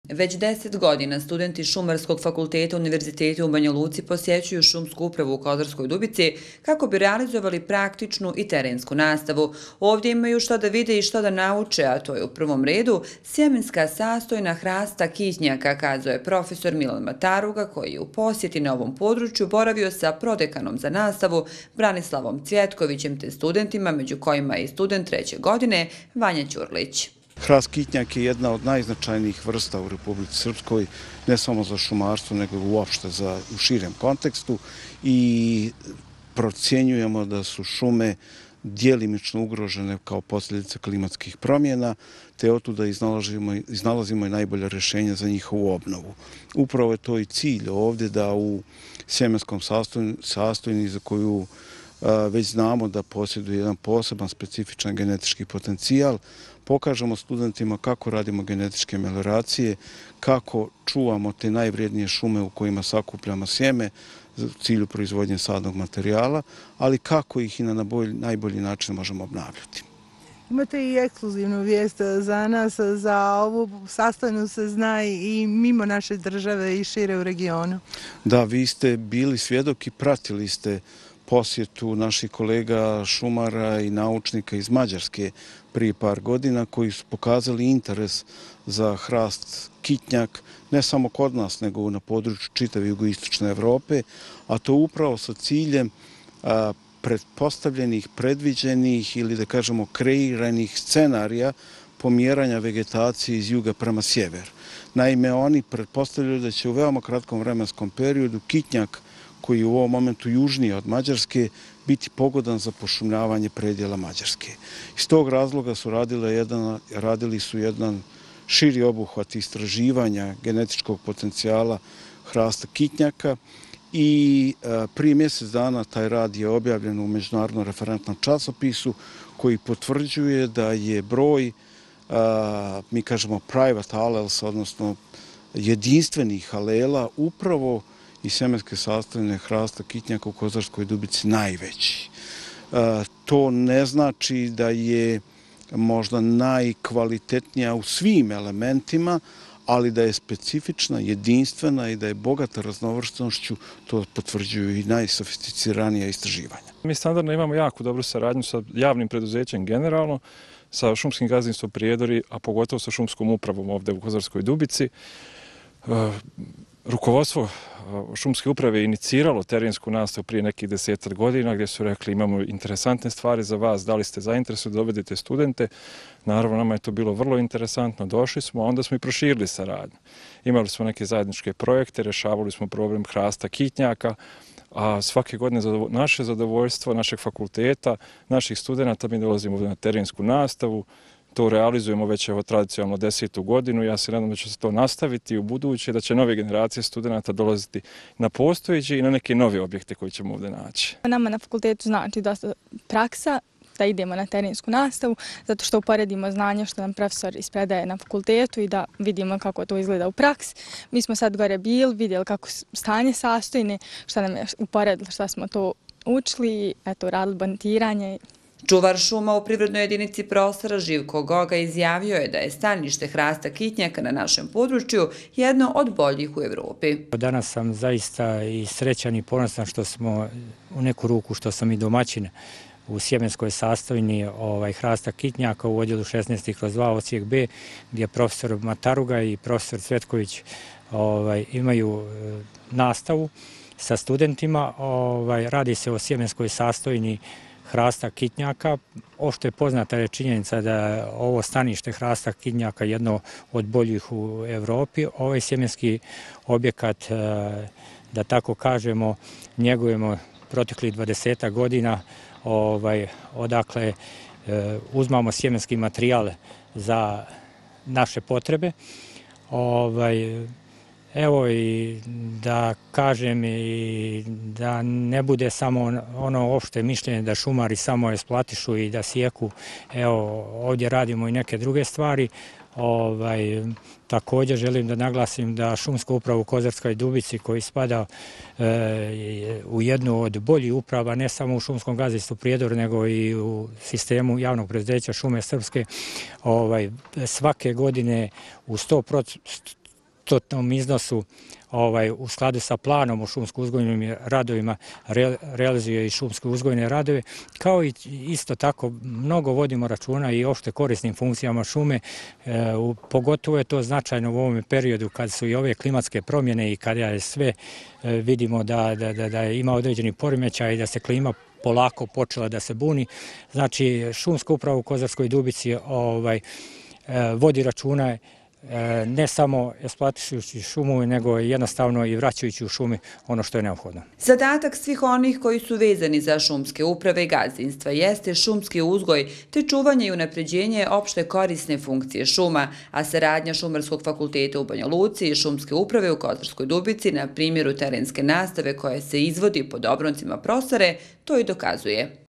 Jubilarno deseto izvođenje praktične nastave za studente u ŠU Kozarska Dubica
Već deset godina studenti Šumarskog fakulteta Univerziteta u Banjaluci posjećuju Šumsku upravu u Kozarskoj Dubici kako bi realizovali praktičnu i terensku nastavu. Ovdje imaju šta da vide i šta da nauče, a to je u prvom redu sjemenska sastojina hrasta kitnjaka, kazao je profesor  Milan Mataruga koji je u posjeti na ovom području boravio sa prodekanom za nastavu Branislavom Cvjetkovićem , te studentima među kojima je i student treće godine Vanja Ćurlić.